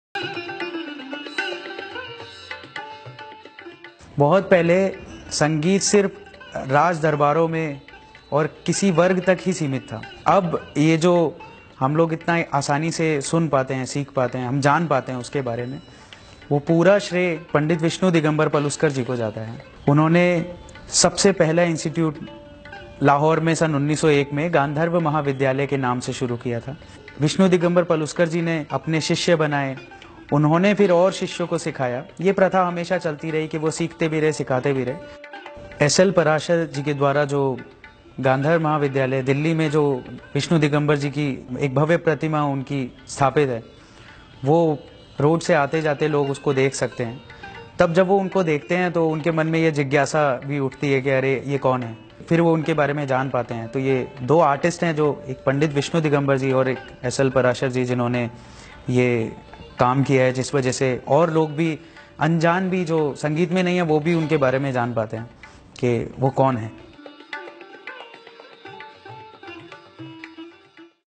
Saint Musician || Pandit Vishnu Digambar paluskar || Musical Journey
Speaker Artist -Khushal Sharma ( shastriye vocalist)
Statue Creator - S.L.Parashar